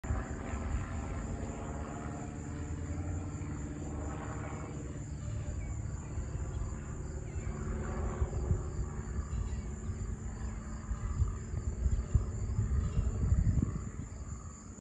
Airplane - 8/14/22
Here is a video of an airplane in Goldsboro, Pennsylvania on Sunday, August 14, 2022. Enjoy!